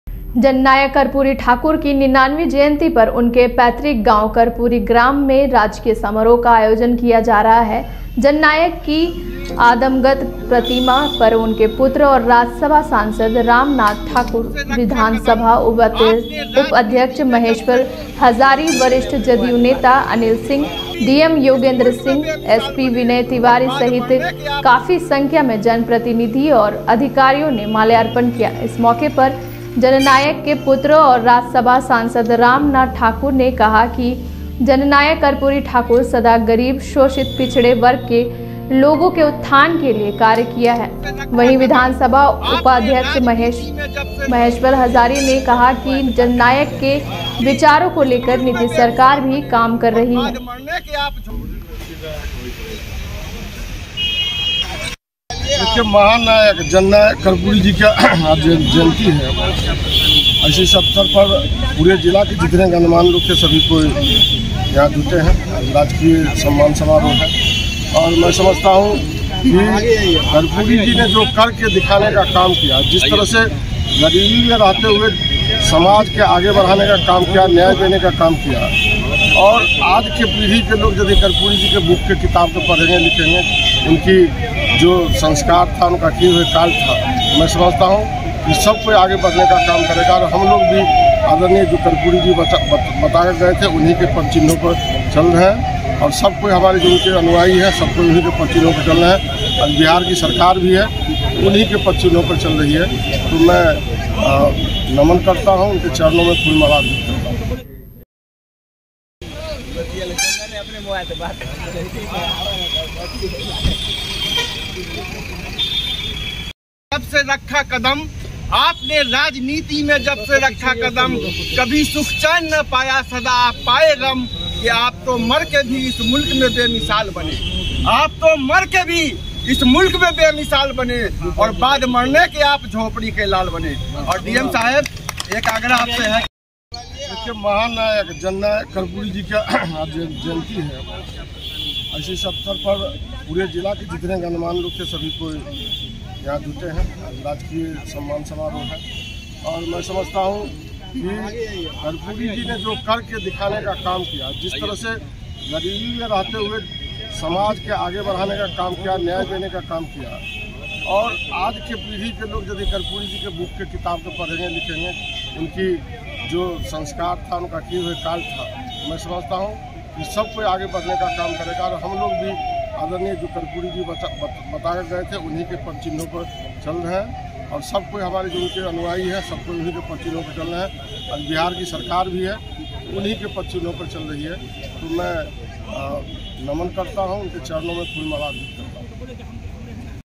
पैतृक गांव कर्पूरीग्राम में राजकीय समारोह के साथ मनाई गई, जननायक कर्पूरी ठाकुर की 99 वीं जयंती
22Scope Live TV | 22 Scope | Hindi News | 22 Scope लाइव